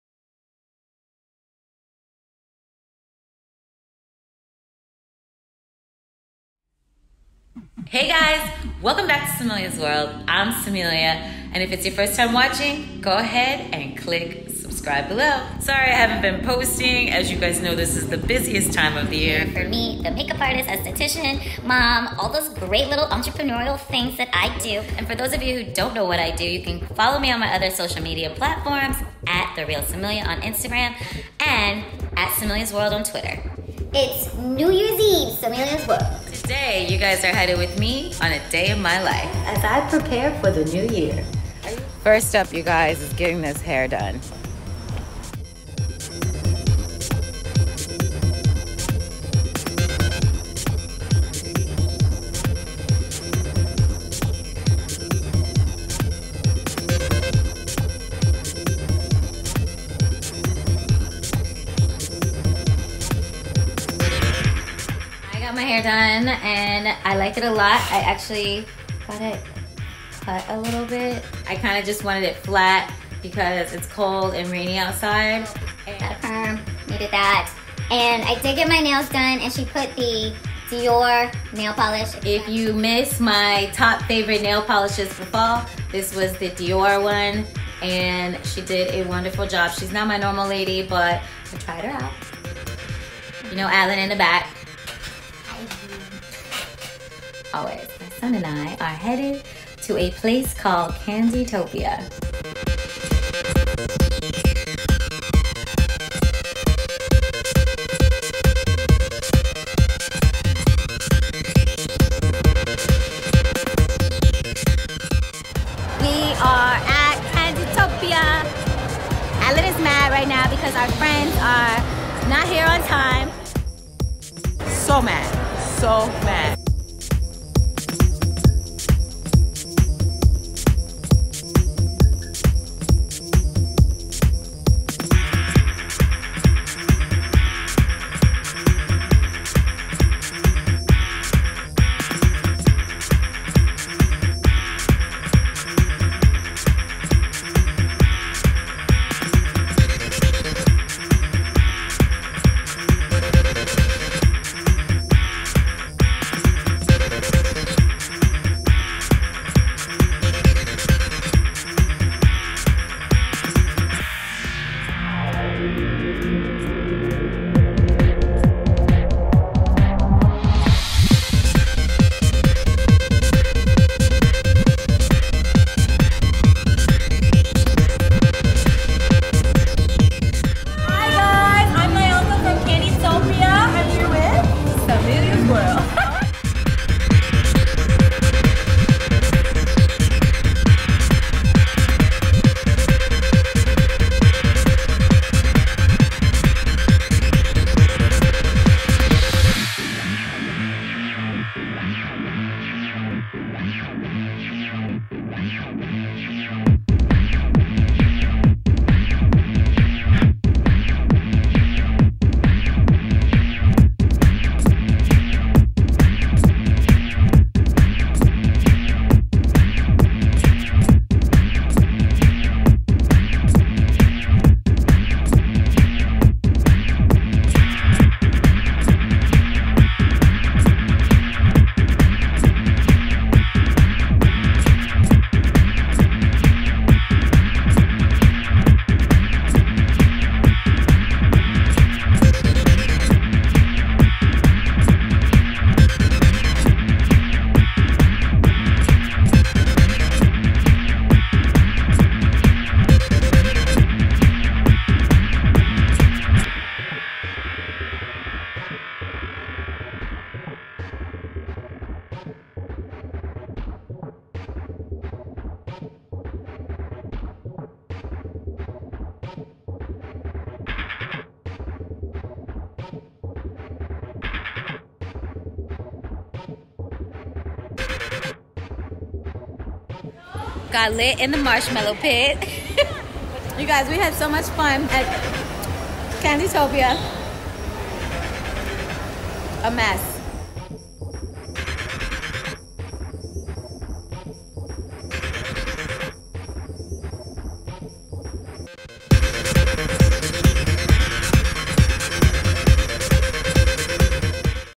NEW YEAR's EVE VLOG | A DAY IN MY LIFE
SAMELIA's WORLD today is the last day of the year and I hope you all enjoy it as much as I am! Happy NewYEARsEve ... I wish all of you nothing but the best this year and positivity throughout the next year to come! Let all continue to support and show love to each other everyday! Be safe tonight and I hope you all enjoy this video.

Youtubes every Tuesday and Thursday